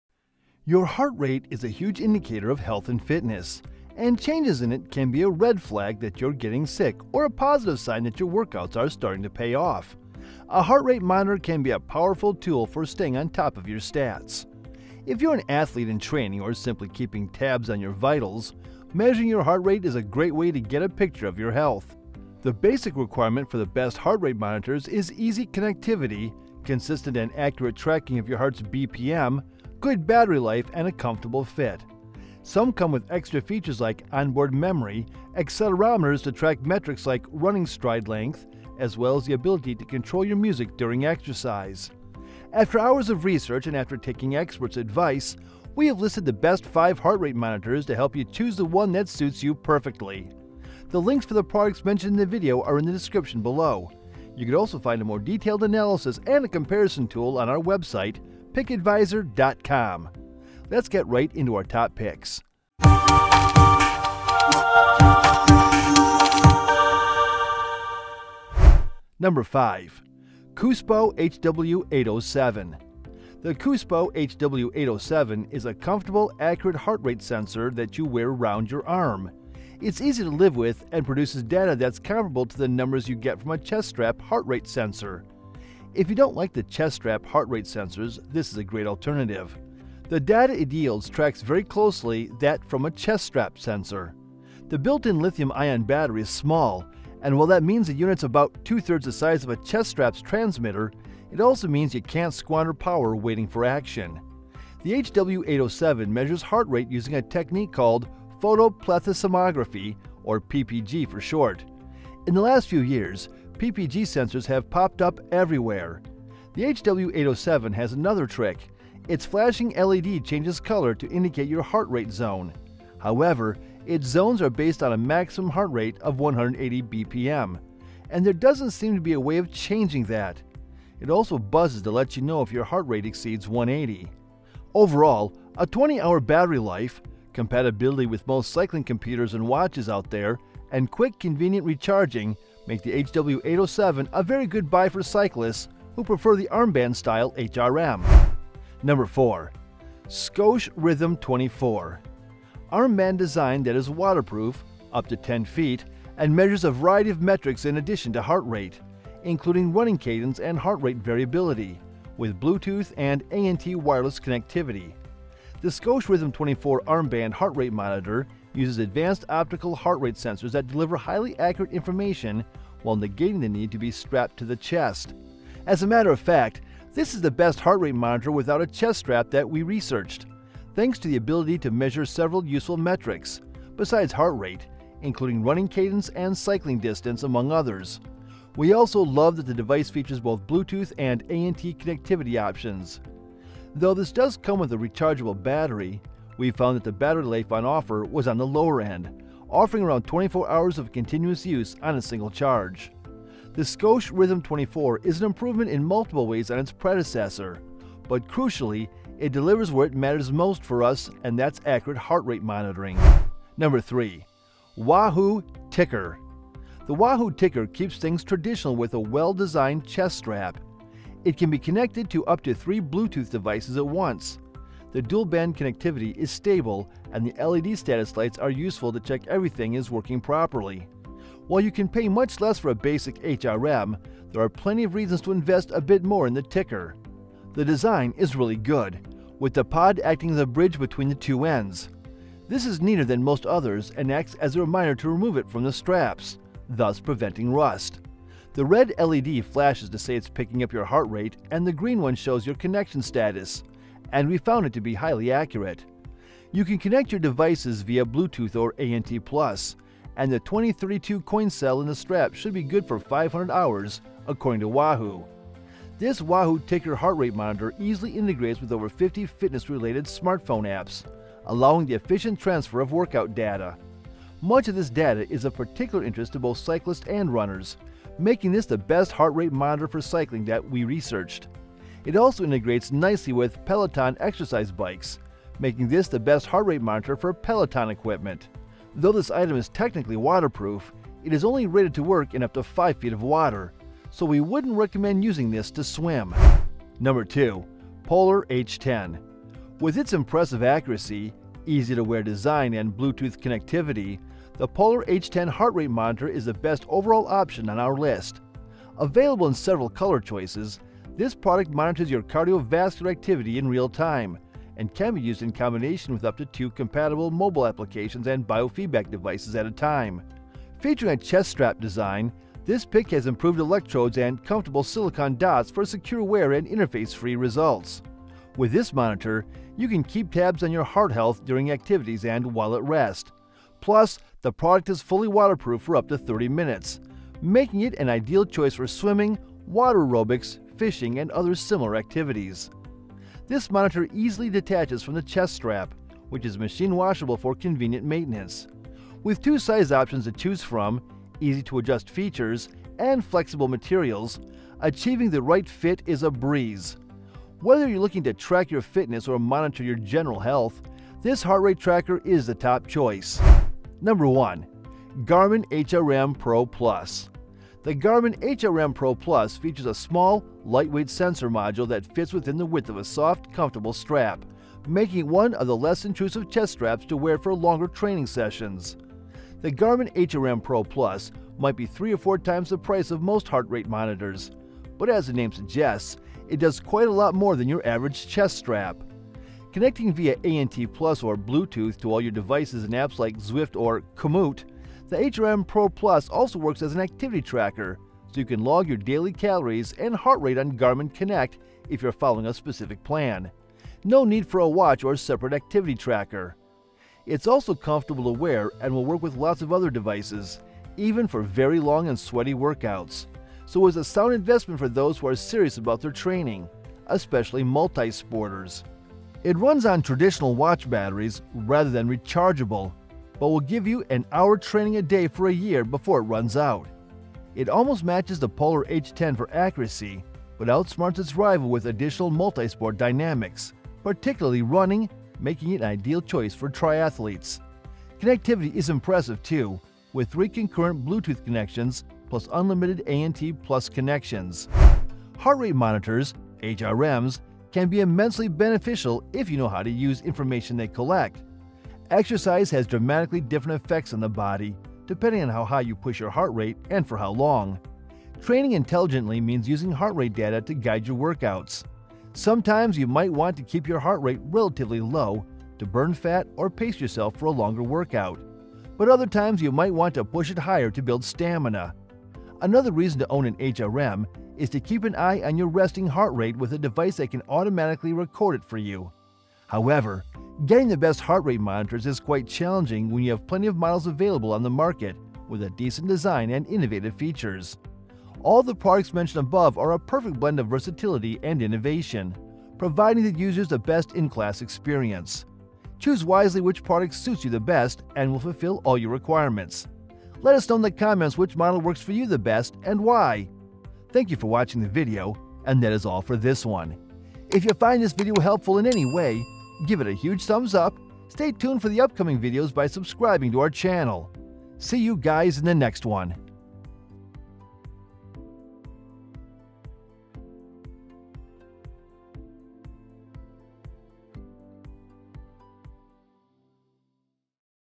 Best Heart Rate Monitors 2025 - The Only 5 You Should Consider Today!
Best Heart Rate Monitors Shown in This Video:

5. COOSPO HW807
4. Scosche Rhythm 24
3. Wahoo TICKR
2. Polar H10
1. Garmin HRM Pro Plus

0:00​​​ -  Introduction
1:09​​​ - 5. COOSPO HW807
2:29​​​ - 4. Scosche Rhythm 24
3:34 - 3. Wahoo TICKR
4:58​​​ - 2. Polar H10
6:04​​​ - 1. Garmin HRM Pro Plus
7:26​​​ - Conclusion

Best heart rate monitors (HRMs) can be immensely beneficial if you know how to use the information they collect. Exercise has dramatically different effects on the body depending on how high you push your heart rate and for how long.
Training intelligently means using heart rate data to guide your workouts. Sometimes you might want to keep your heart rate relatively low to burn fat or pace yourself for a longer workout, but other times you might want to push it higher to build stamina. Another reason to own the best heart rate monitor is to keep an eye on your resting heart rate with a device that can automatically record it for you.
The basic requirement for the best heart rate monitors is easy connectivity, consistent and accurate tracking of your heart's BPM, good battery life, and a comfortable fit. Some come with extra features like onboard memory, accelerometers to track metrics like running stride length, as well as the ability to control your music during exercise. So, here's what we know about the Best Heart Rate Monitors in 2025.